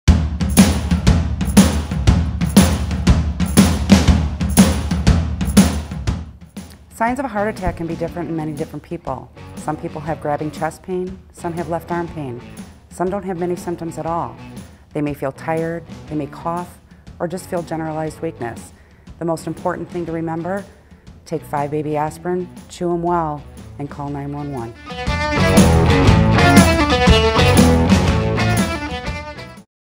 On The Fly: Chest Pain
On The Fly quick tip is from Illinois' Chief Flight Nurse Keri Brach, CFRN. OTF Quick Tip deals with chest pain and some of the ways a heart attack could present. Make sure you always call 911 in case of an emergency.